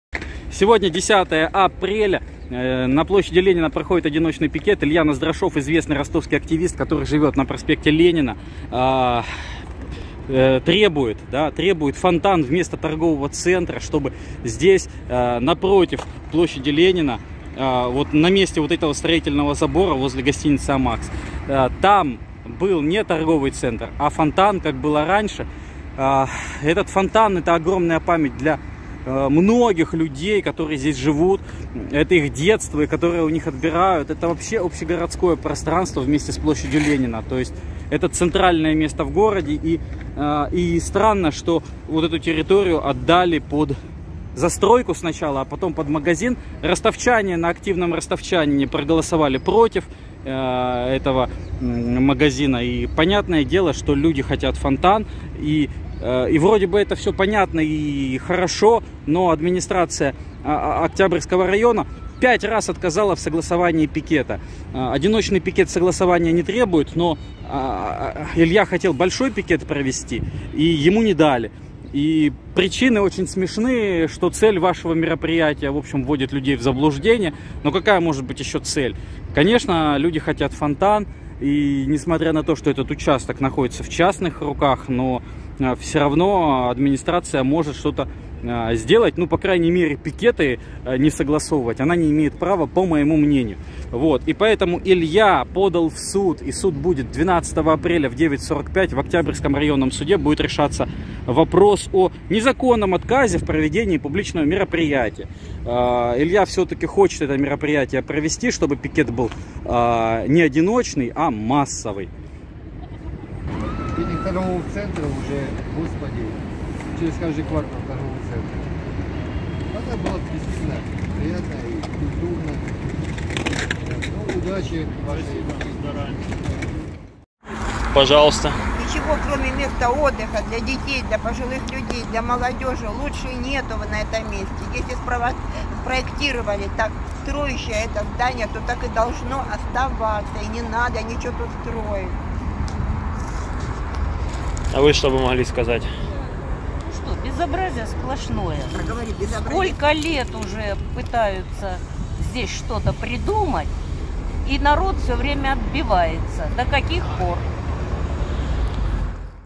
Пикет за фонтан не согласовали пять раз!
Ростовчанин Илья Наздрашов вышел с одиночным пикетом «Мы за фонтан». Илье уже пять раз не согласовали массовый пикет, и поэтому он вышел один. Одиночный пикет не требует согласования.

На сайте Активный Ростовчанин горожане высказались против строительства торгового центра на месте фонтана на площади Ленина. Для многих людей это память.

В понедельник 12 апреля в 9-45 в Октябрьском районном суде будет рассматриваться дело о незаконном несогласовании пикетов. Илья Наздрашов всё-равно хочет провести массовый пикет и просит ростовчан его поддержать.

Илья Наздрашов уже организовывал пикеты на сохранение ростовского троллейбуса и о возврате троллейбусных маршрутов на проспект Ленина, и тогда власти пошли навстречу.